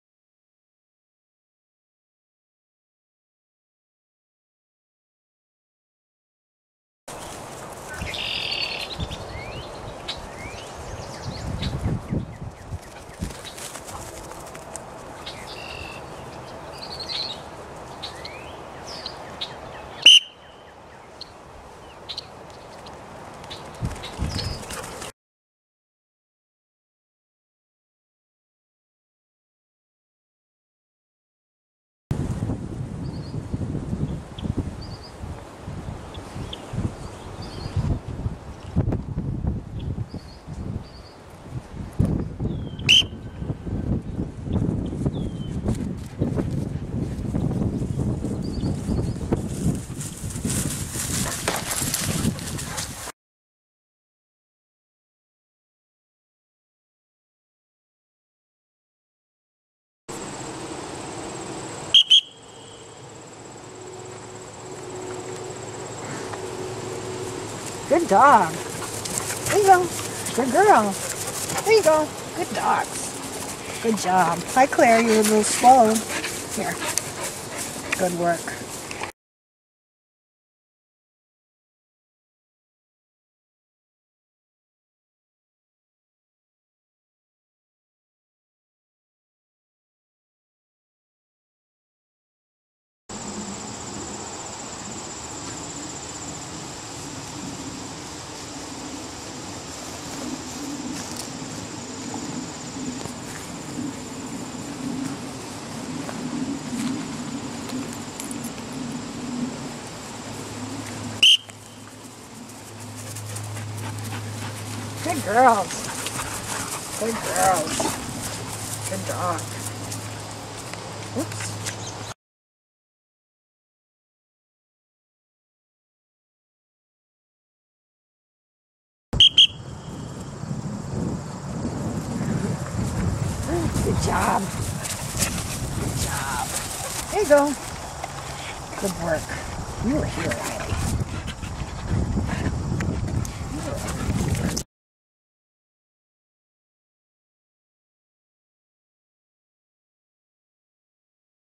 Dog Training: Whistle Recalls
Chinooks Intervale Claire, Belle and Jazzy and Belgian Tervuren Sky demonstrate the power of the whistle for distance recalls.

To train the whistle recall, just pair the sound of the whistle with the delivery of tasty treats along with a round of praise to the good dog(s) who come to you.  Start close and build up to distance.  One loud short blast is all you need!

Breeders can teach pups to associate the whistle with food while in the whelping box or, in just a few sessions, dogs will easily learn the whistle recall while running and playing with other dogs who are whistle savvy.

My Chinooks have taught Belgian Tervs the whistle recall in just one session!  (Of course, everyone knows that Tervs are brilliant! :)